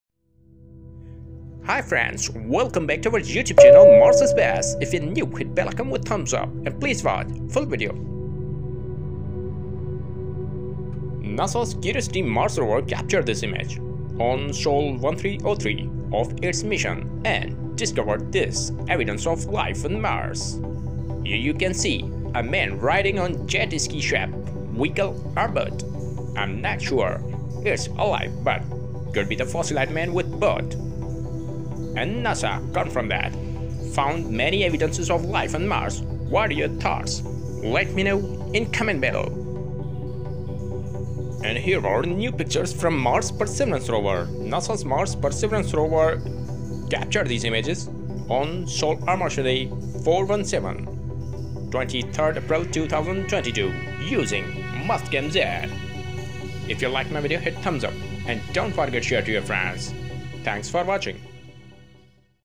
Martian Boat: Life on Mars | Jet Ski boat on Mars spotted by NASA's Curiosity Rover | Perseverance
Martian Boat: Life on Mars | Jet Ski boat on Mars spotted by NASA's Curiosity Rover | Perseverance Rover

In this video we have shown a jet ski boat on Mars spotted by Curiosity Rover with latest images of mars from NASA's Mars Perseverance Rover of its mission.

Credit: NASA.GOV
NASA/JPL-Caltech